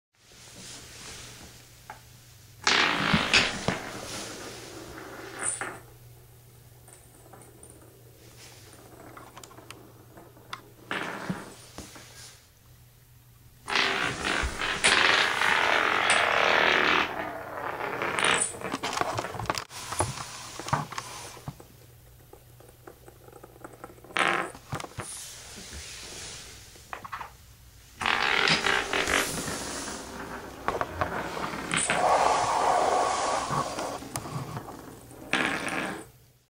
Japanese Spinning Tops
A short demonstration of a wooden top that spins upside-down if it goes fast enough.